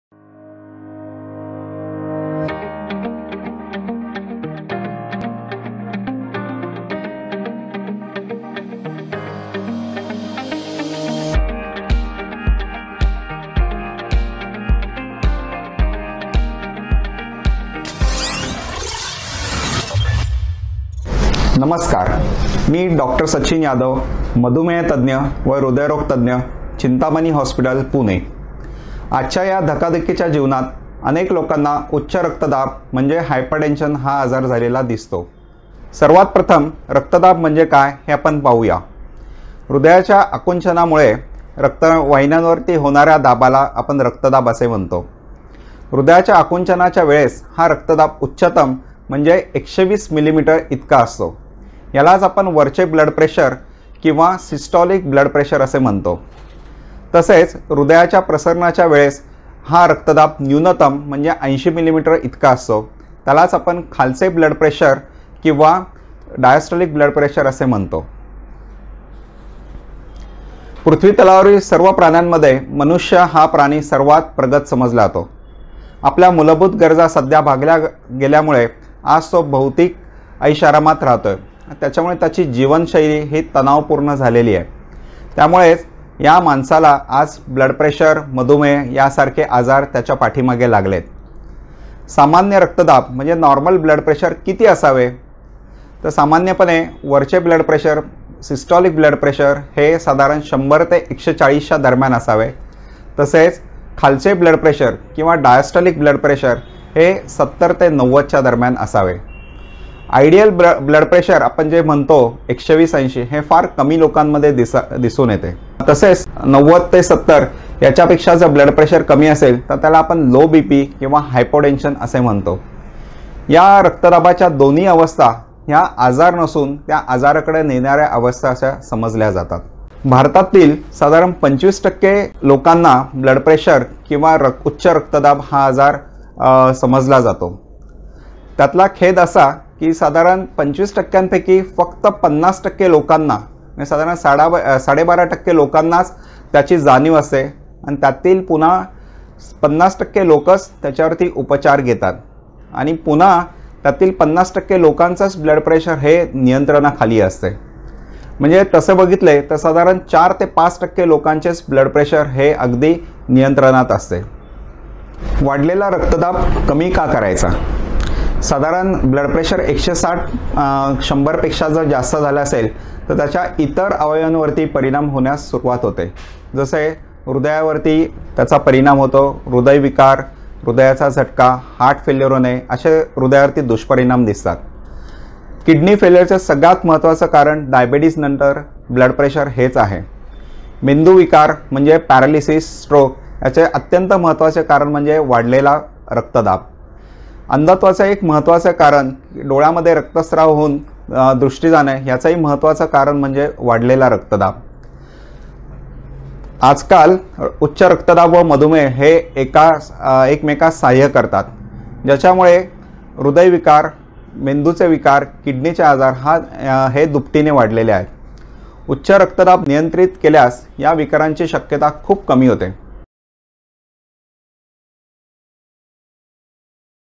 Understanding Blood Pressure (Marathi)
Hypertension is the chronic state of uncontrolled blood pressure. Dr. Sachin Yadav, general physician has given concise information on blood pressure, normal value, side-effects and prevalence of blood pressure. Also, he gives a brief on importance of reducing high blood pressure and why is it necessary to reduce increased blood pressure in Marathi.